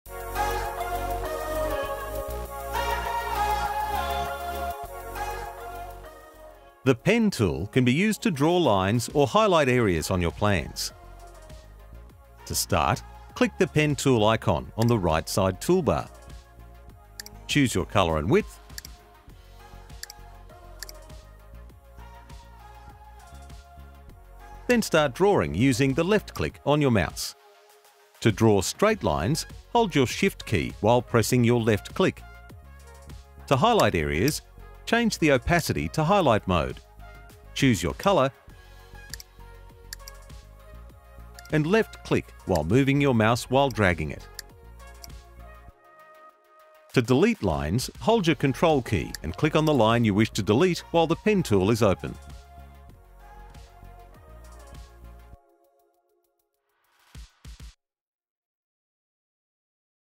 Pen Tool | Groundplan how-to
Use the Pen tool to add a straight line or free-hand annotations on the plan.